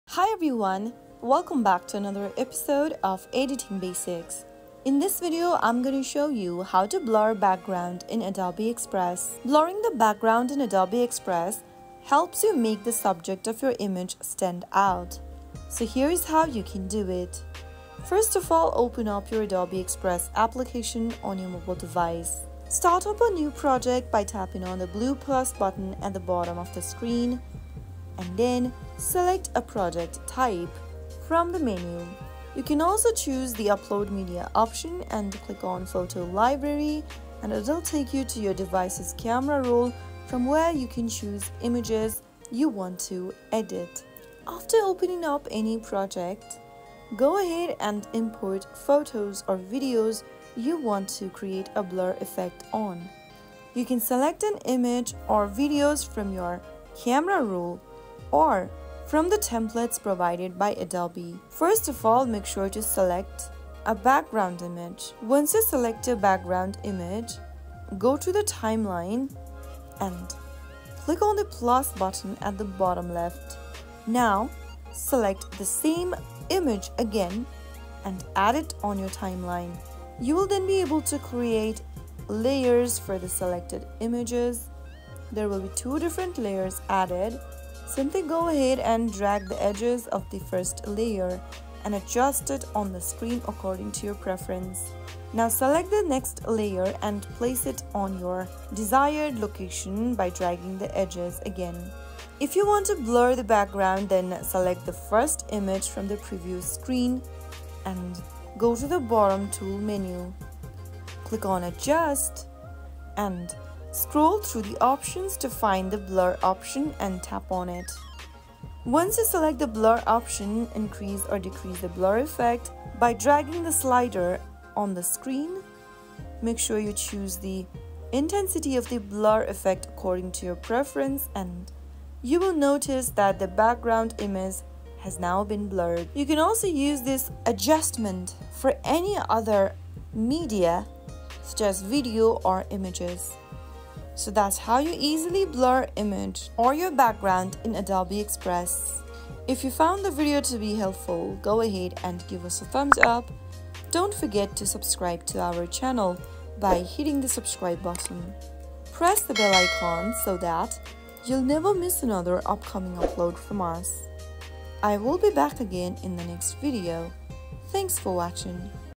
How to Blur Background in Adobe Express 2024?
In this tutorial, we’ll walk you through the process of blurring the background in Adobe Express to make your main subject stand out.

Perfect for enhancing photos, graphics, and promotional materials, blurring the background can add a professional touch to your designs.